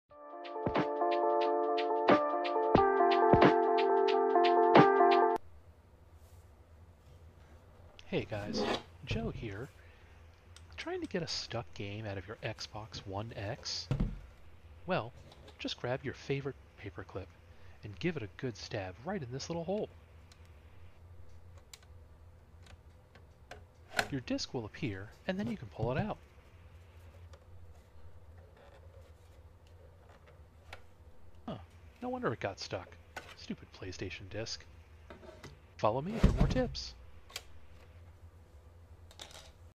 Xbox One X stuck disc
Getting your disc out of your xbox one x is pretty easy.
I'll be releasing the full breakdown of this xbox this week.